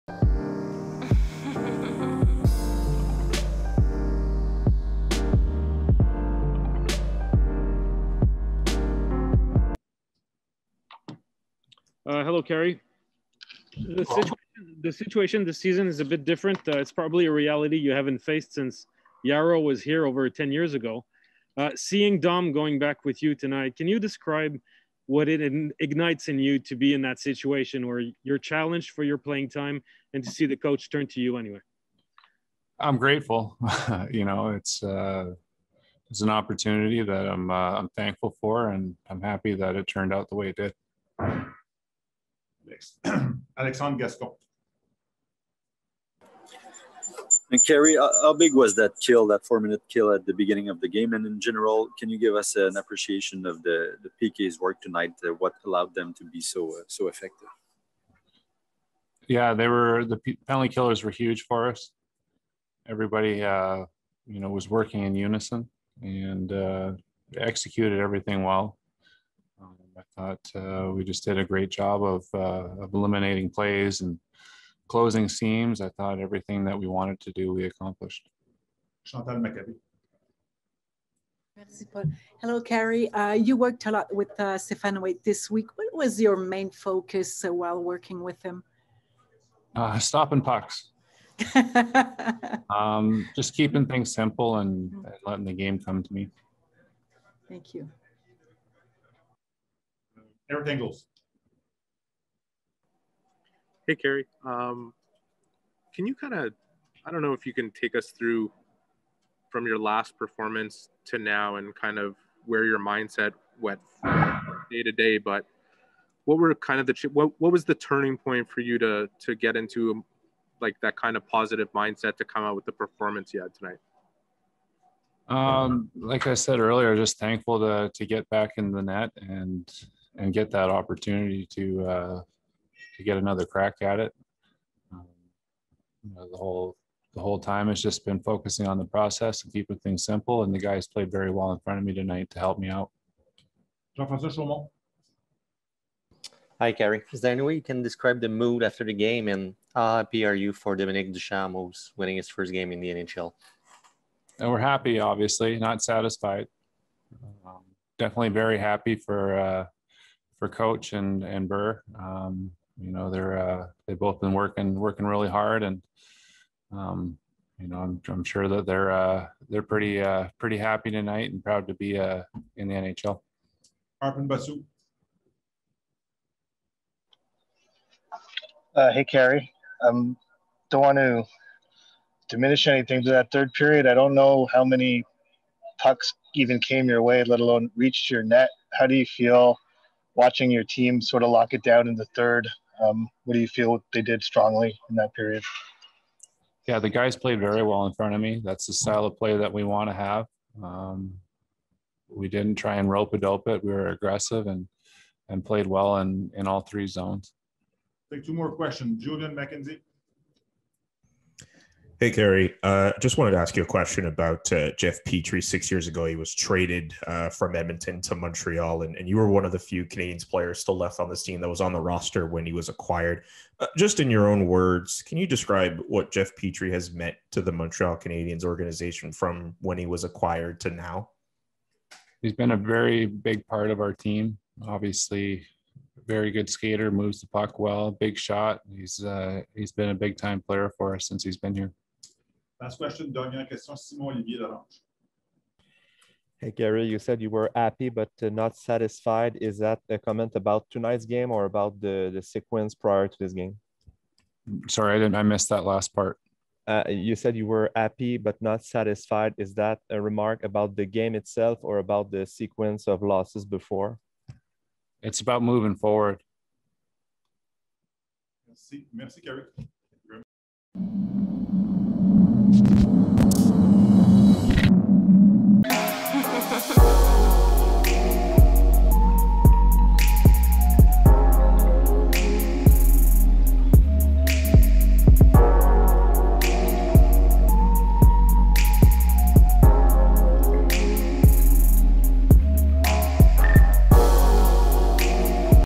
Carey Price Happy for Dominique Ducharme First Win & "Moving Forward" From Rough Games
Watch as Carey Price Happy for Dominique Ducharme First Win & "Moving Forward" From Rough Games